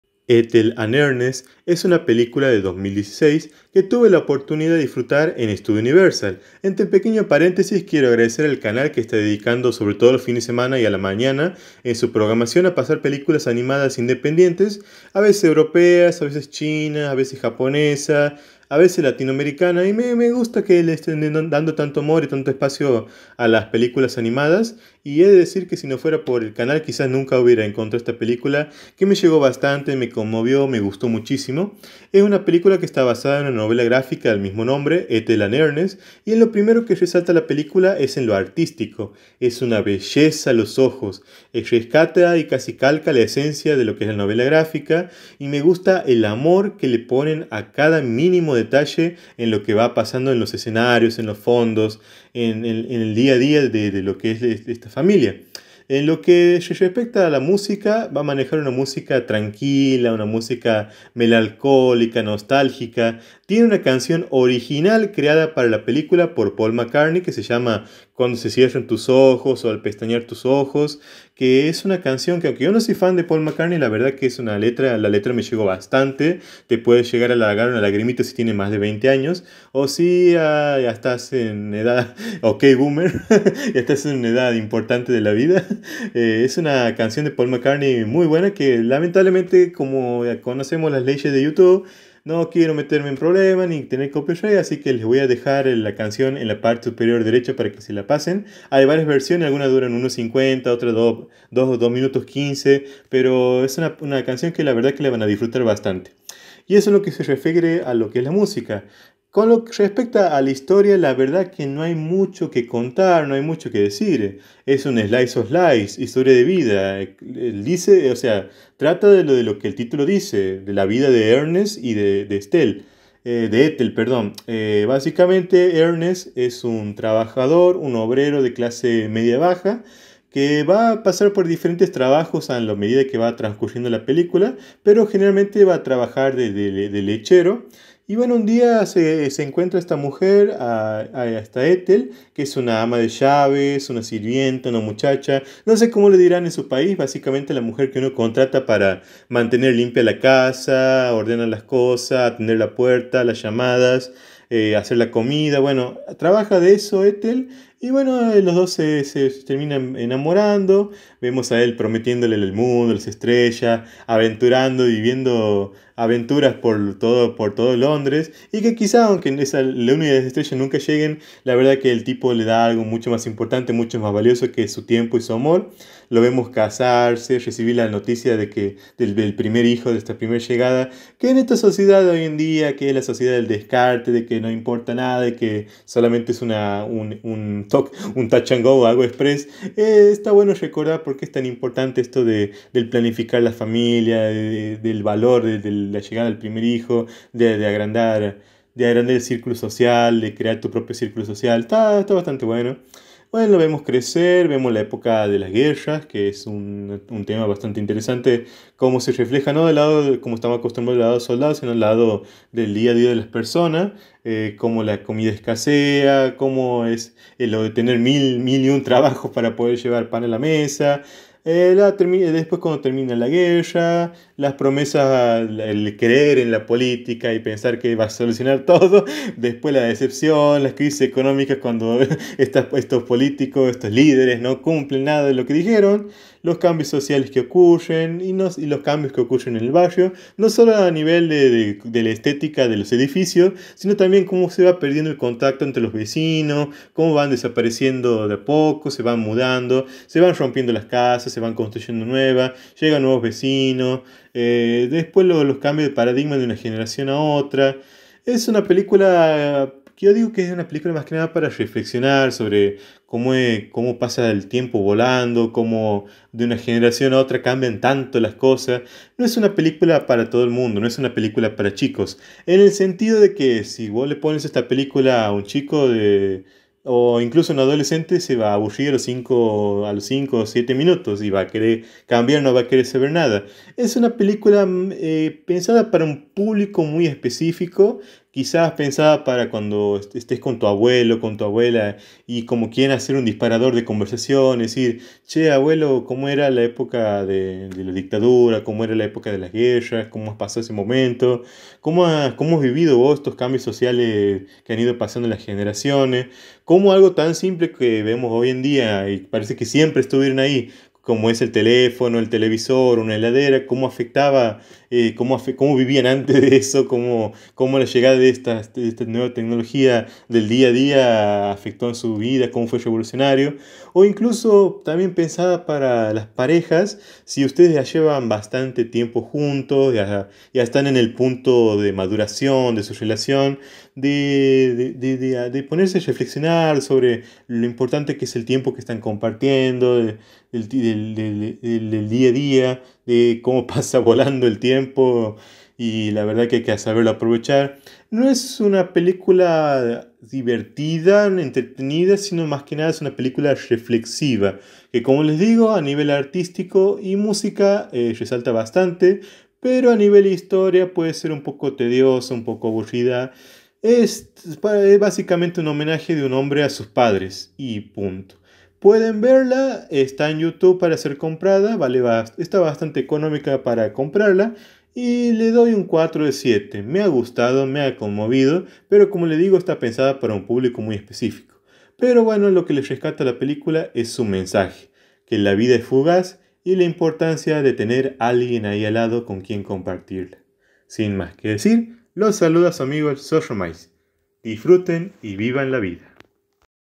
Ethel and Ernest | Reseña
El zorro se pone en modo reflexivo y nos habla de una película que nos recuerda la importancia de pasar la vida con quien se ama.